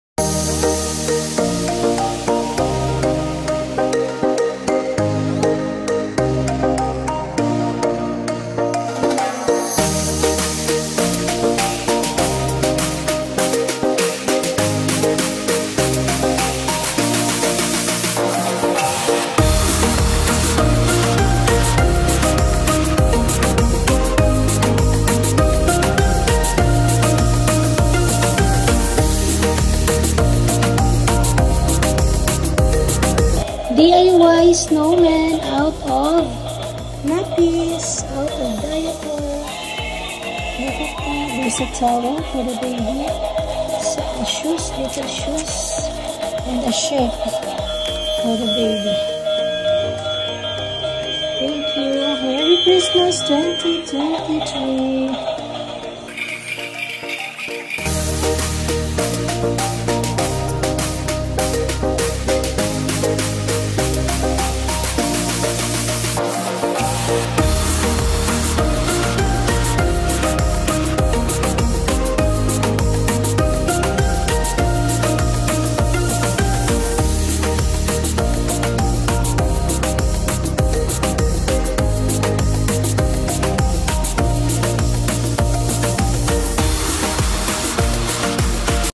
Our baby in the tummy in 21 weeks; size like a pomelo; 24-26cm; 270-350g on 147th day of pregnancy
We thank The Lord for this greatest of gifts. ...A little miracle from above, a beautiful baby to cuddle and love is on the way. Our baby is now 21 weeks; size like a pomelo; 24-26cm; 270-350g on 147th day of pregnancy. grateful blessed babyontheway littlemiracle FirstPregnancyAt47 age47FirstTimePregnancy ThankYouFather LiezelsVentures Ramon Menguito